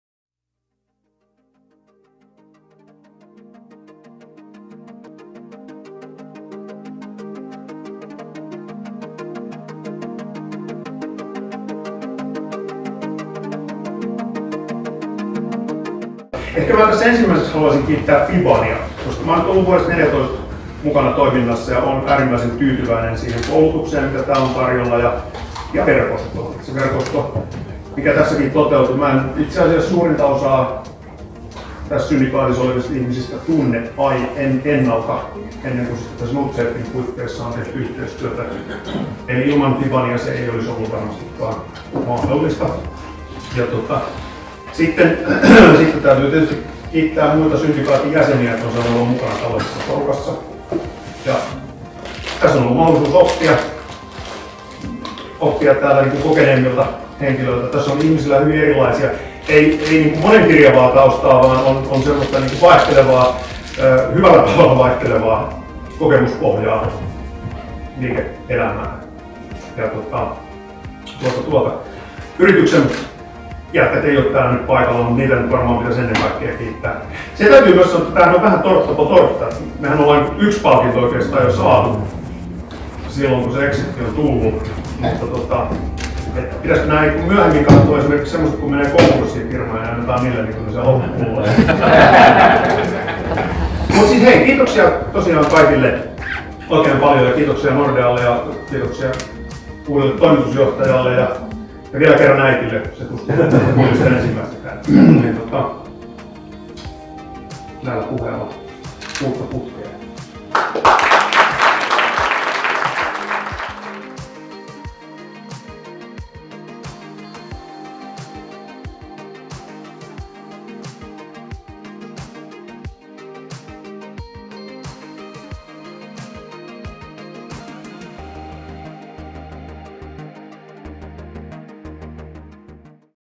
BISNESENKELI-EXIT 2017
Vuoden 2017 Bisnesenkeli-exit -palkinto on myönnetty seitsemän bisnesenkelin muodostamalle yhteissijoitukselle eli sijoittajasyndikaatille. Syndikaattiin kuuluvat Toni Mattila, Tom Lindgren, Anna-Kaarina Lipsanen, Maarit Gockel, Jussi Heinilä, Jukka Hienonen ja Jukka Mäkinen. Bisnesenkeleiden syndikaatti sijoitti työturvallisuuden älykästä mobiiliratkaisua tuottavaan yritykseen NordSafety Oy, josta syndikaatti irtautui marraskuussa 2017 kun osakekauppa allekirjoitettiin saksalaisen EcoIntense GmbH:n kanssa. Yhdessä EcoIntensen kanssa NordSafety voi viedä eteenpäin teknologista kehitystä työturvallisuuden osalta sekä vauhdittaa kasvua entisestään.